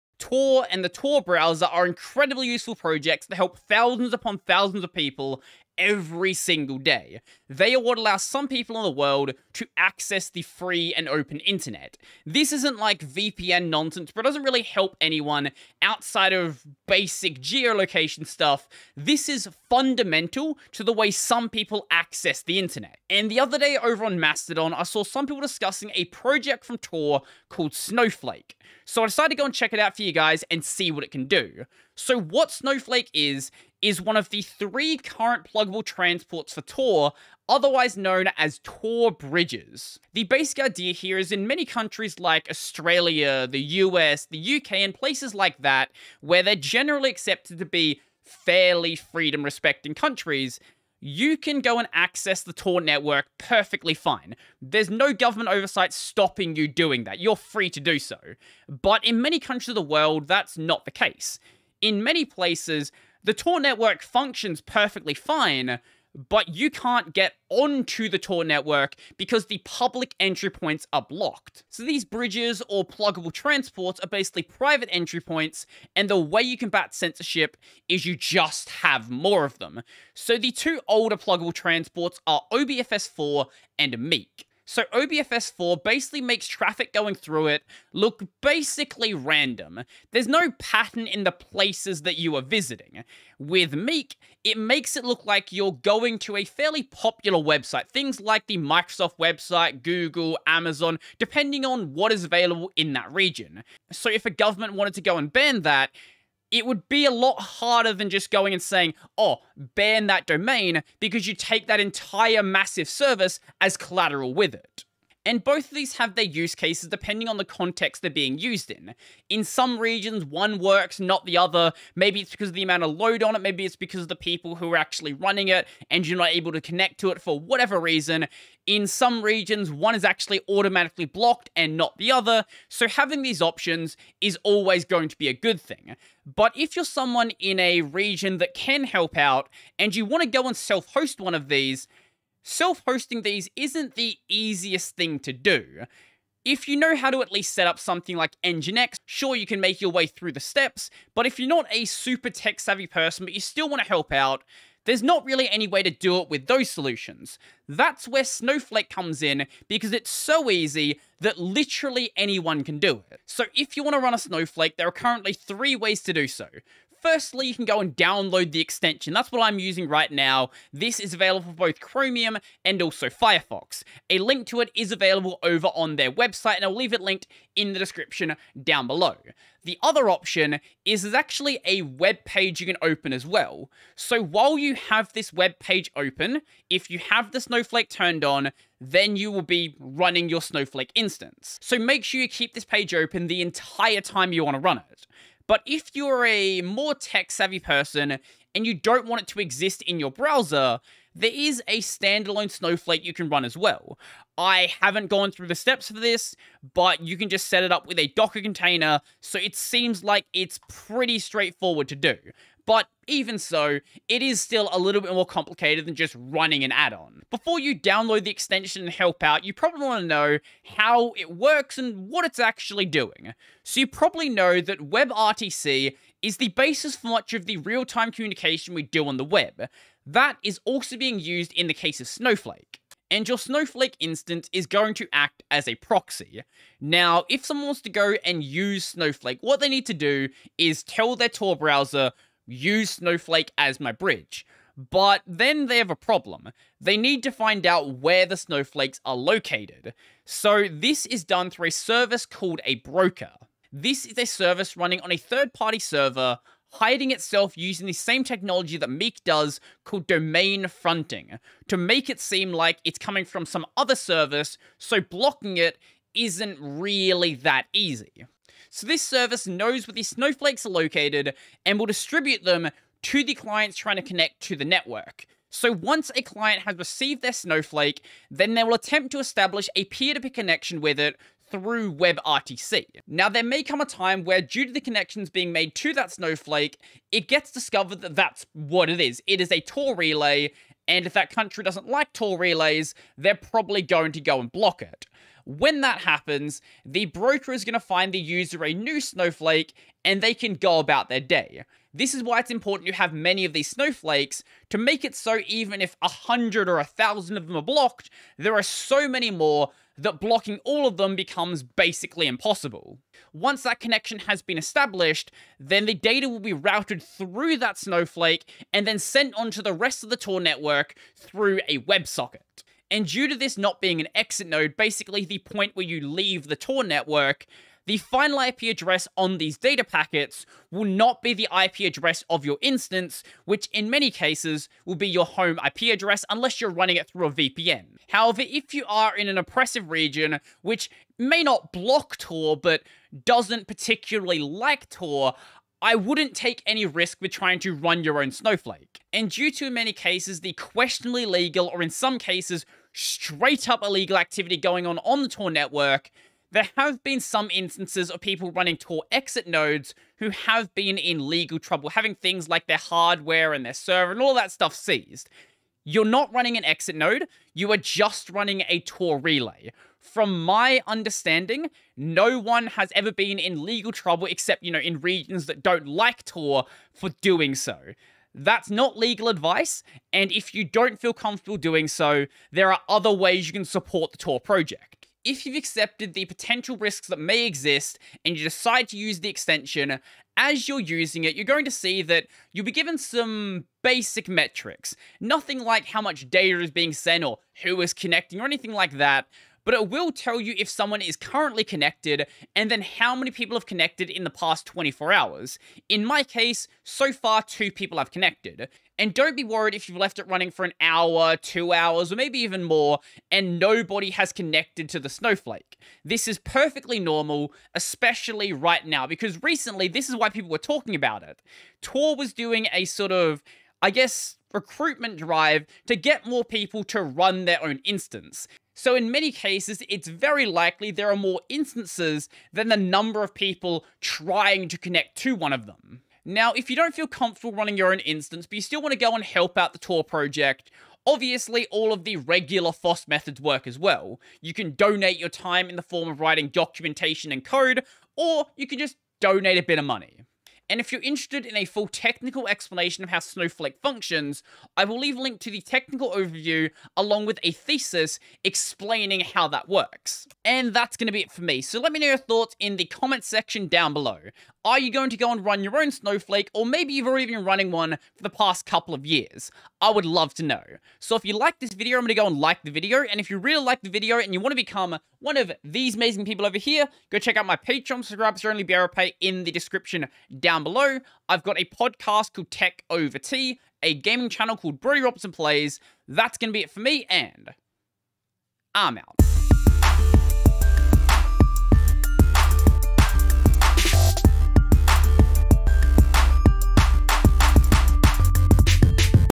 Become A Tor Snowflake And Fight Censorship
For some people in the world the only way they can reasonably access the internet is through Tor but to ensure the it's usefulness it requires many volunteers and snowflake makes running a TOR relay node easier than ever.

Time Stamps
0:00 Introduction
0:39 What Is Snowflake
2:43 How To Run Snowflake
3:37 How Snowflake Works
7:30 Outro

Credits
🎨 Channel Art:
All my art has was created by Supercozman